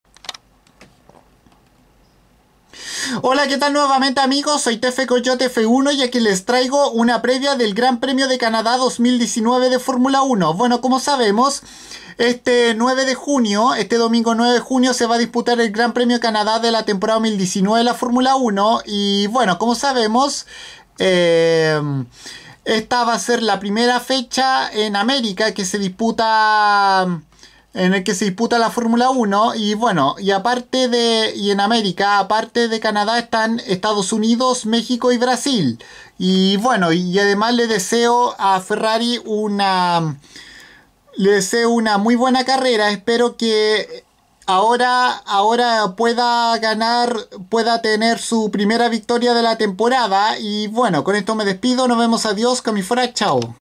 Previa GP de Canadá 2019 de F1
Una breve previa de la primera carrera americana de la Temporada 2019 de la Fórmula 1 que se correrá en el circuito semipermanente de Gilles Villeneuve en Montreal Isla de Ille Notre Dame